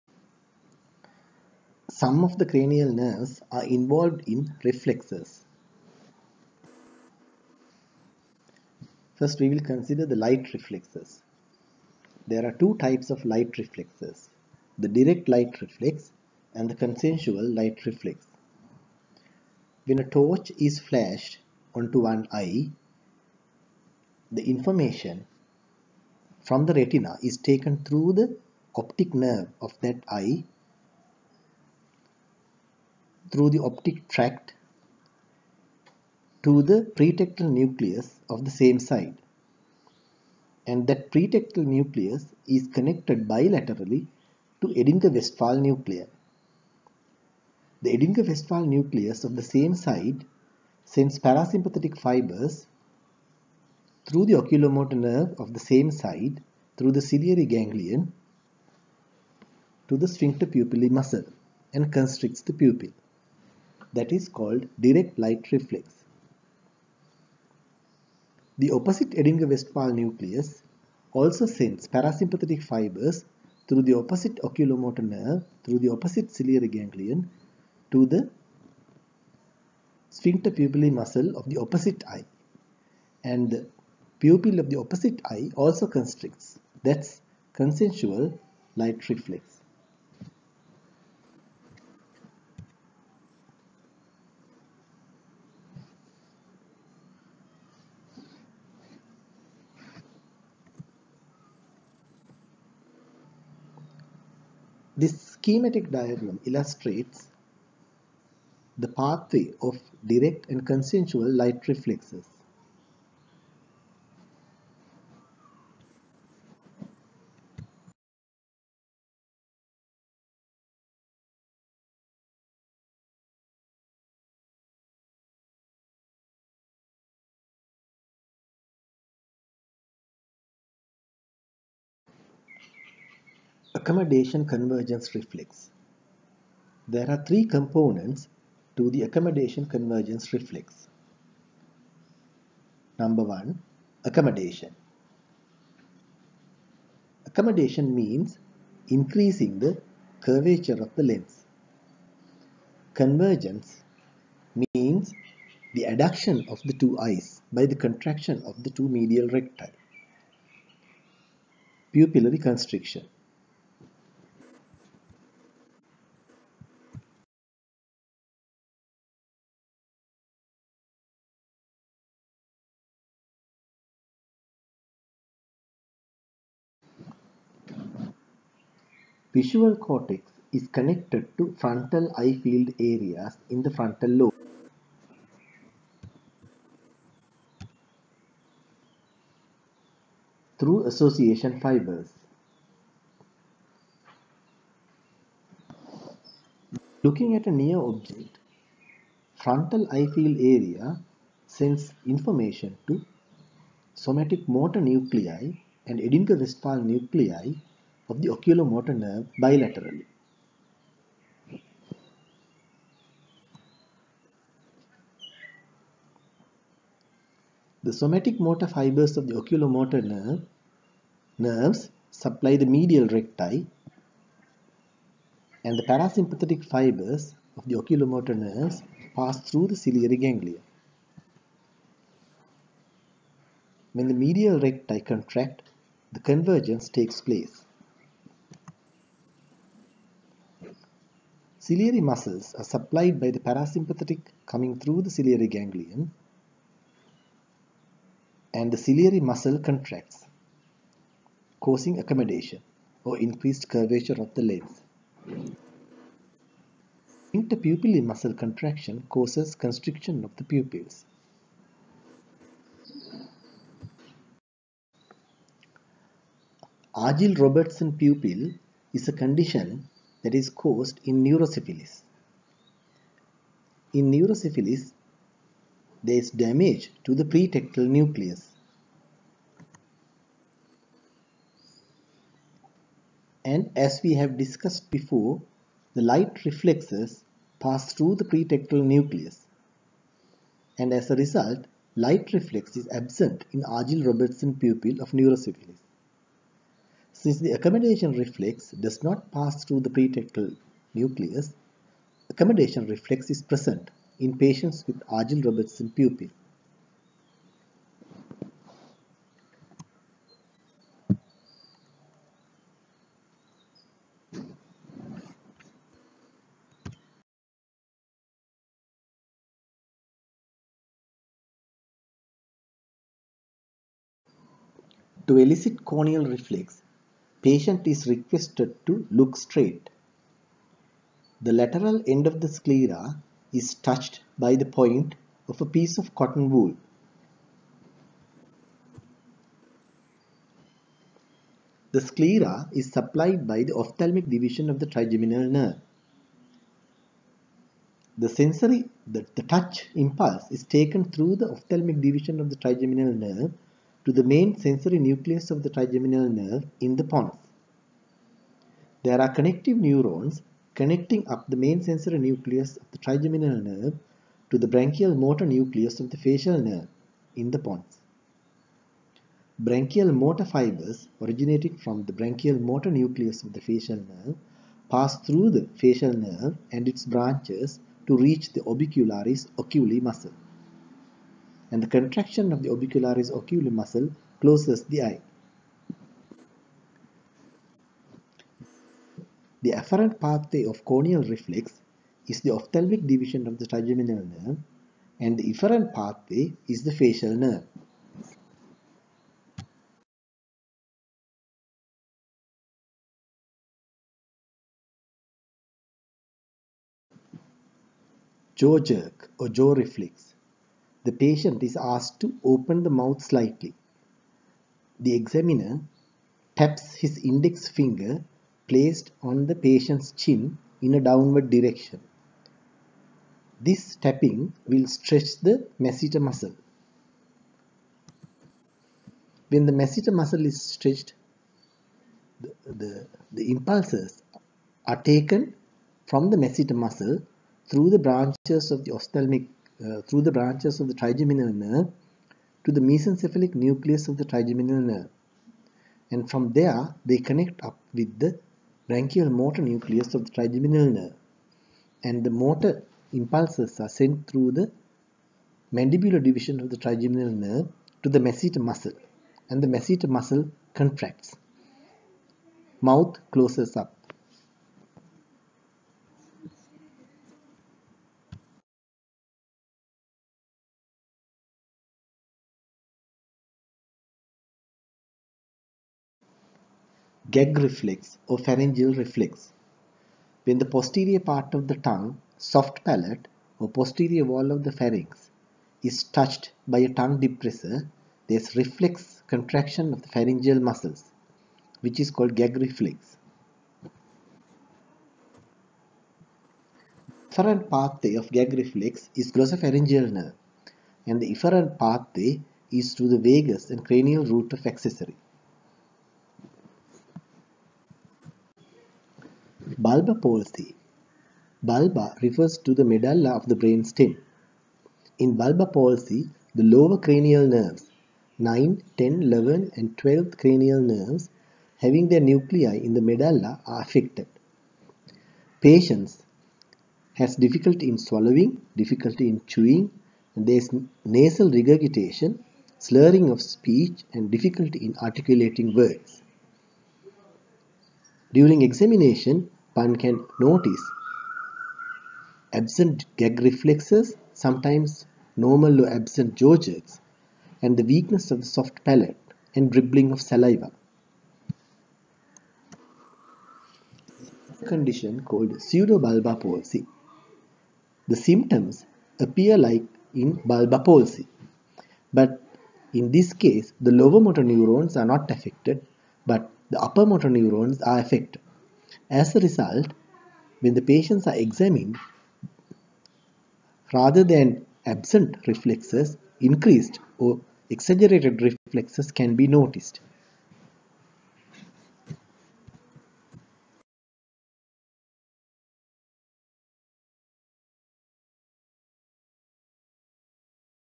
Cranial nerve reflexes
This short video is on the pathways of main cranial nerve reflexes. Afferent and efferent pathway are discussed.